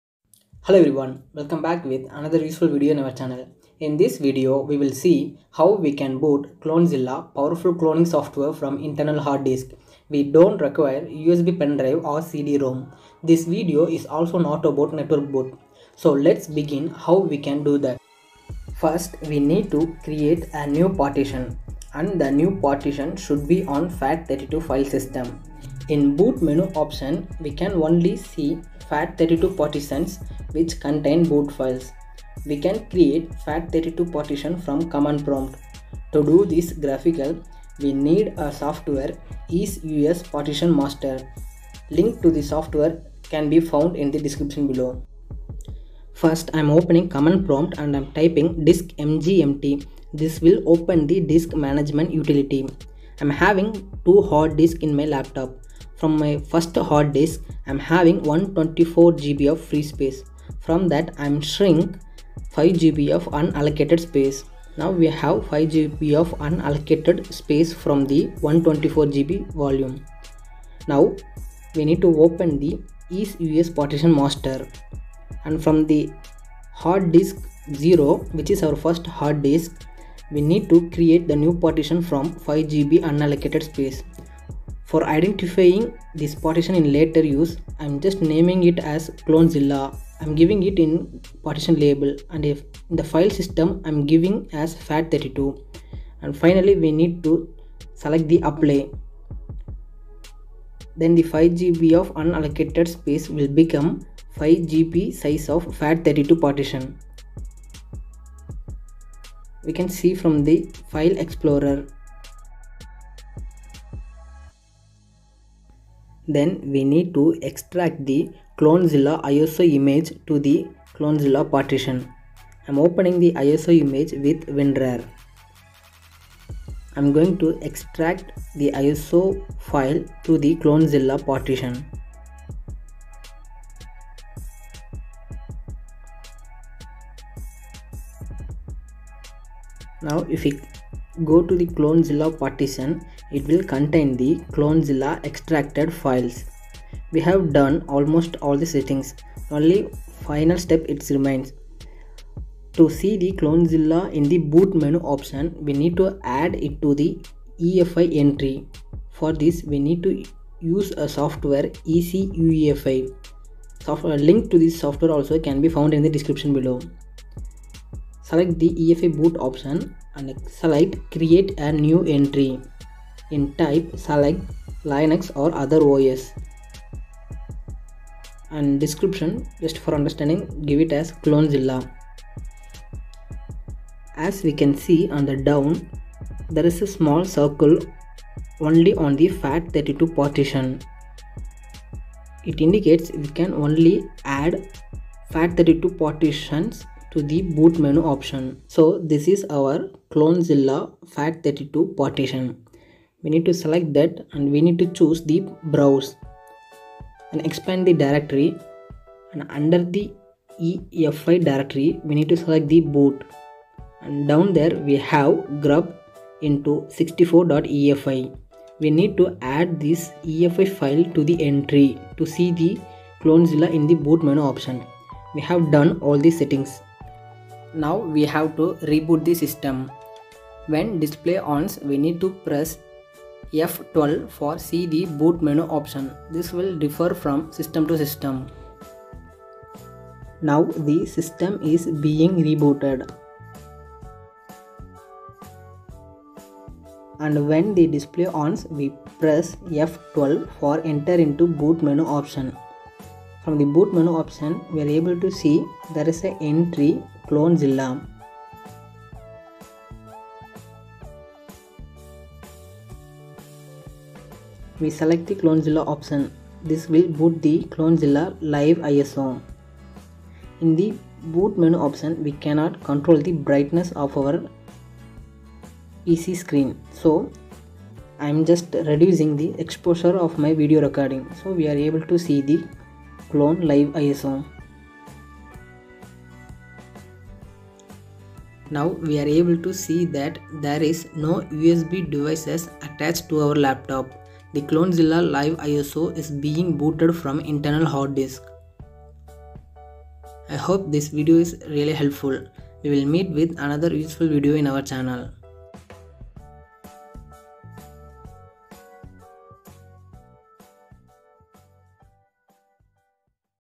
Boot Clonezilla from internal hard disk
We use Clonezilla software for cloning operating systems and restore them. Cloning is very handy method to save lots of time we spend for changing operatings system. In this video we will see, how we can boot Clonezilla from internal hard disk. By booting Clonezilla from internal hard disk, we don't need USB pendrive. Booting Clonezilla from internal hard disk also saves resources we need to restore cloned OS such as second computer, pendrive, LAN cable in terms of network boot.